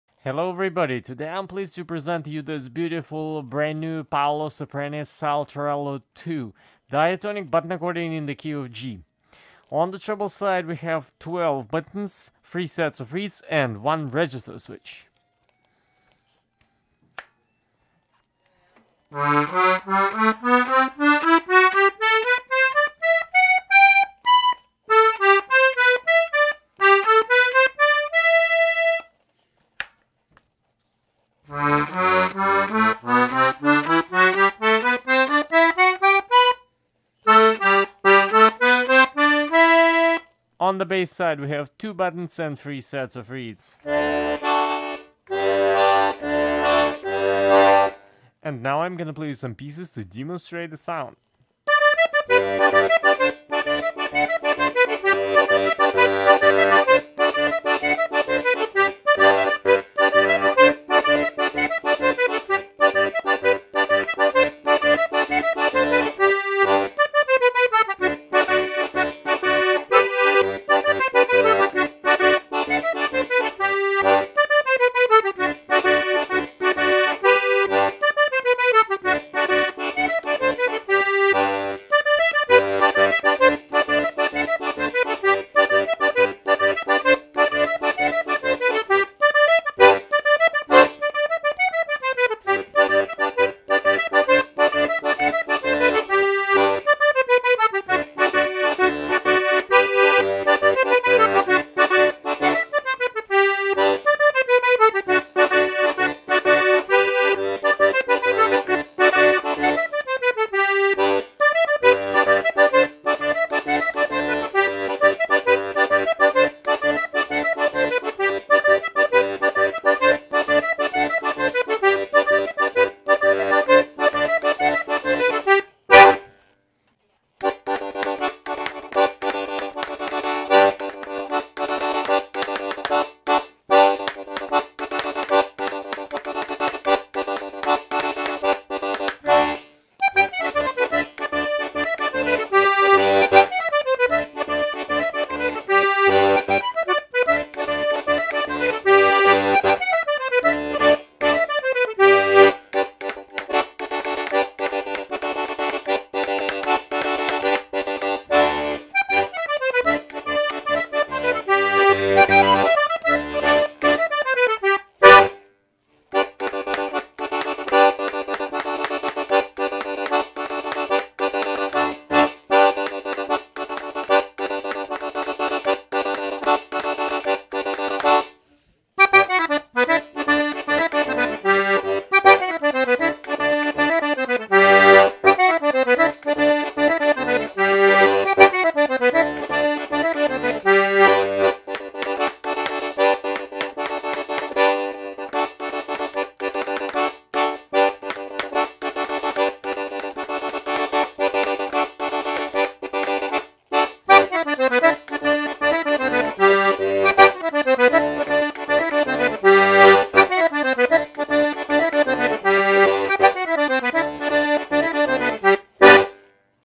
PSSALTGC - Cherry Paolo Soprani Saltarello II Organetto Diatonic Button Accordion GC LMM 12 2 $1499
Brand: Paolo Soprani
Model: Saltarello II
Type: Diatonic Button Accordion
Reeds: 3/3 LMM
Treble: 12 Buttons, 7.5"(19cm) Button to Button, 1 Register
Bass: 2 Bass Buttons, 0 Registers
Weight: 5lbs / 2.3kg
Features: Key of G with reversals, switch between MM and LMM
Includes: New Straps and Case, 1 Year Factory Warranty
Suggested Upgrades: Pro Microphone Installation, Spray Polish